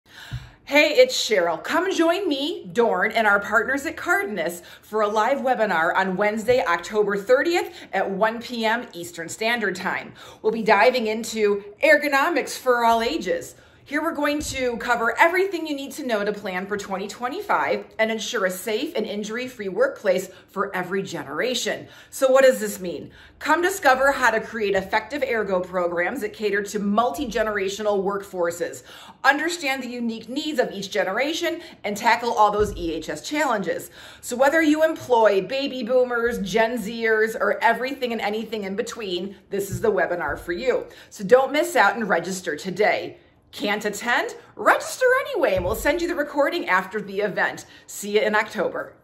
Industry Webinar with DORN and Cardinus: Ergonomics For All Ages
Calling All Safety Professionals!

Join us for a live webinar on October 30th at 1 PM EST to explore the future of ergonomics and injury prevention across a multigenerational workforce with Cheryl Roy and Cardinus Risk Management.

Whether you’re a seasoned safety leader or new to the field, this session will offer fresh perspectives and actionable strategies to elevate your EHS programs.

What to Expect:
1) Discover how to tailor safety programs for all ages and understand the unique needs of five generations working together.
2) Learn about emerging trends and solutions to keep your workforce safe, engaged, and injury-free in the years ahead.
3) Gain practical insights on how to address the top EHS challenges and enhance your safety initiatives with data-driven approaches.

Don’t miss this opportunity to stay ahead of the curve and ensure your safety programs are ready for the future.